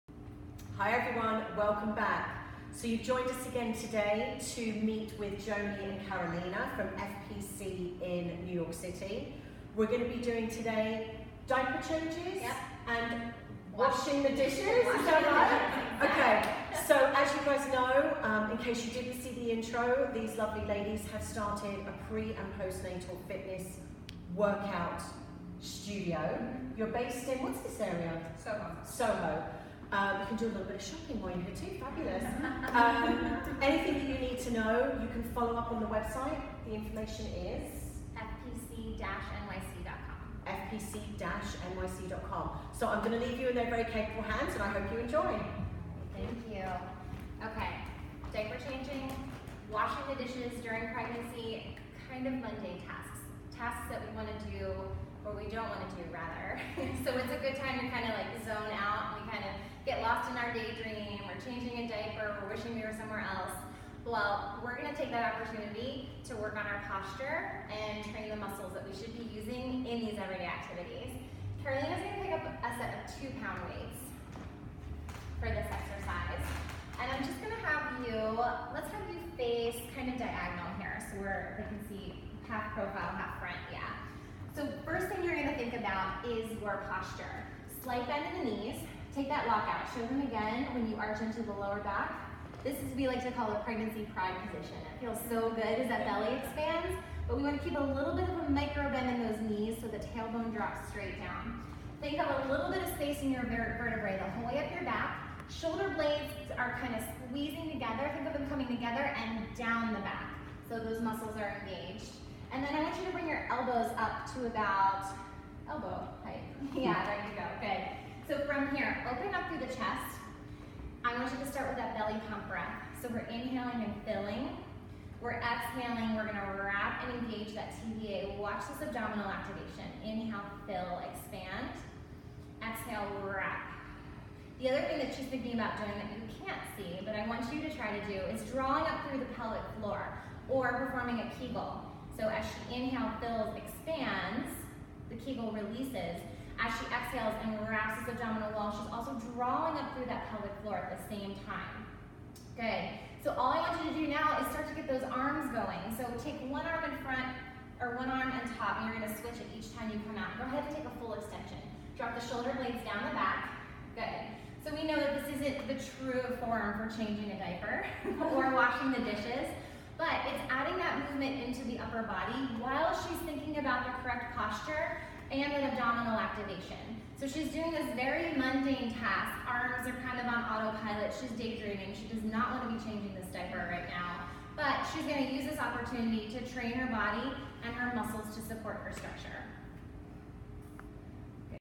TPC & FPC Exercise Series: Diaper Changes and Washing the Dishes
In this series, Joanie and Carolina from Fit Pregnancy Club walk you through exercises to help women stay fit in pregnancy.  Next up - Diaper Changes and Washing the Dishes!